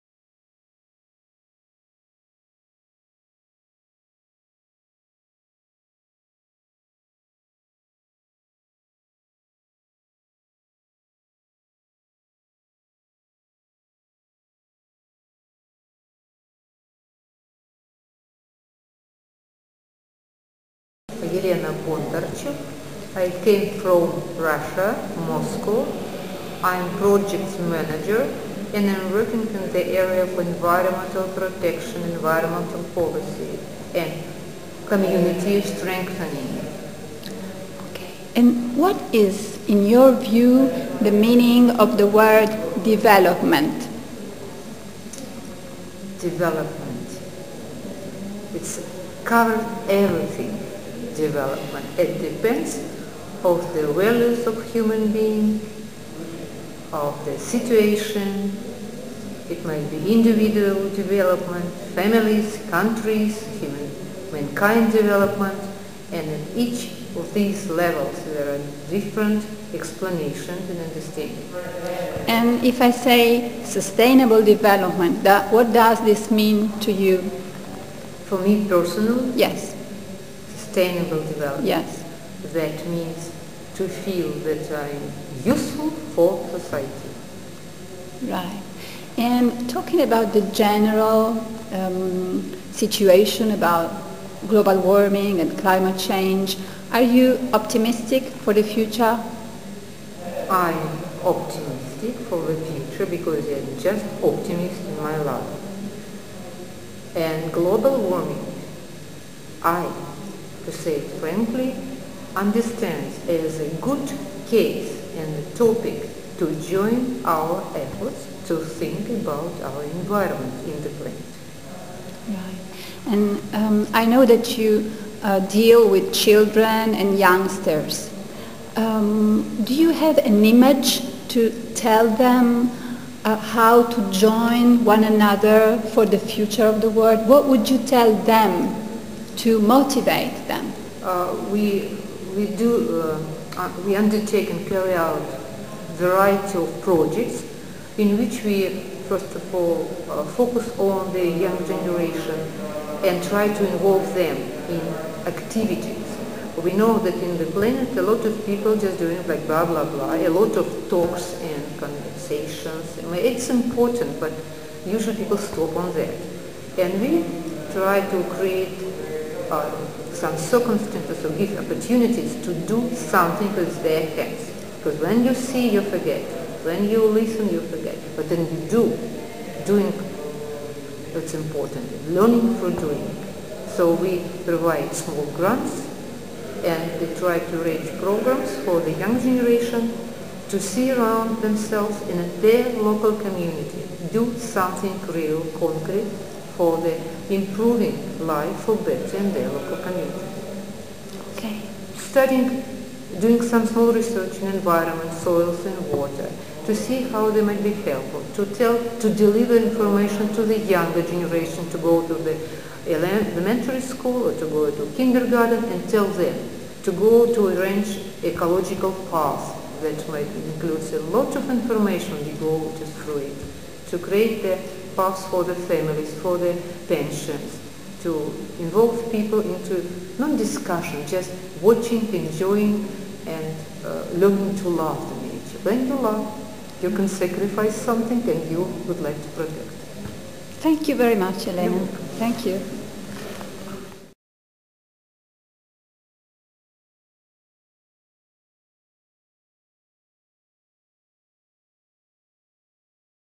Elena A. Bondarchuk, the meanings of sustainible development
Interview to Elena A. Bondarchuk, PhD at Fund for Sustainable Development in Russia, that has been taken during the VI International Conference on Ethics and Environmental Policies which took place in Padova during 23-25 October 2008.
Elena Bondarchuck points out the fact that the meaning of word development cover everythings, from human beings to situation.
We need to bring children into real useful sustainable activities if we want create the opportunities to do something which can't be forgotten.